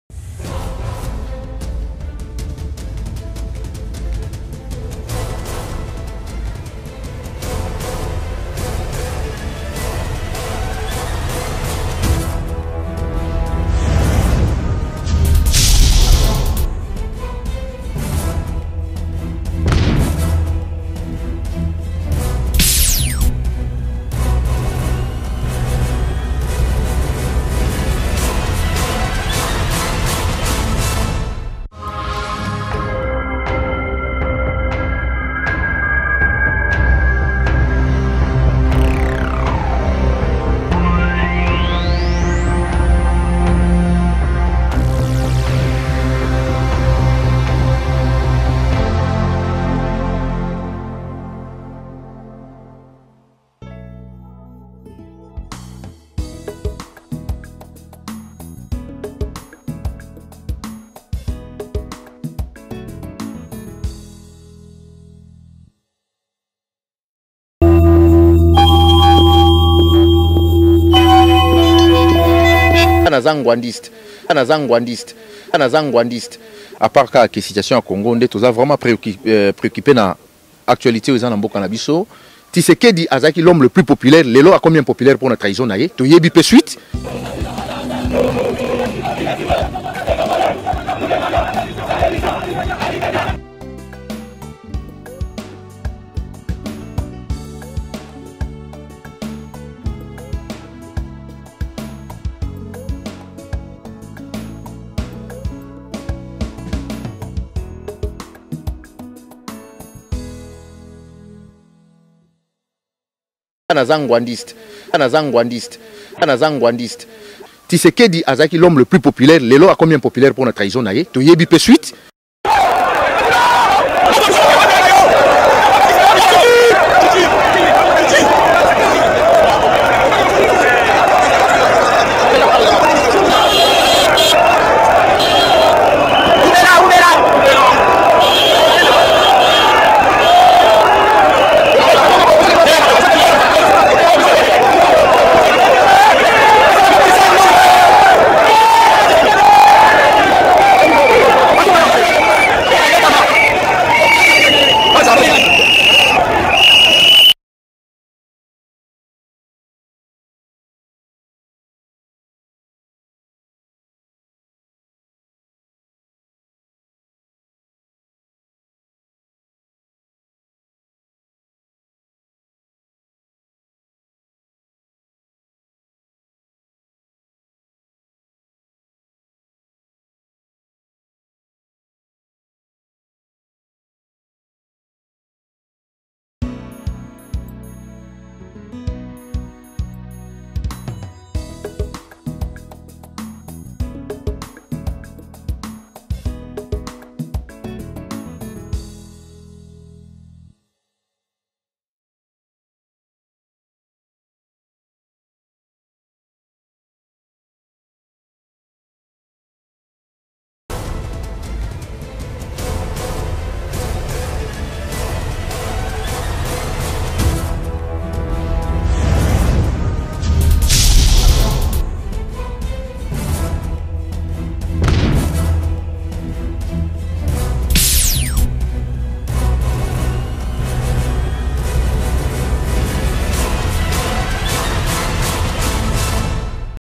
TSHISEKEDI POPULAIRE OU IMPOPULAIRE? QUI DI VRAI ODON MBO OU PEUPLE??
ODON ALOBI TSHISEKEDI AKOMA IMPOPULAIRE NA RDC  QUE DIT IL DE CET ACCEUIL TROP CHALEUREUX?.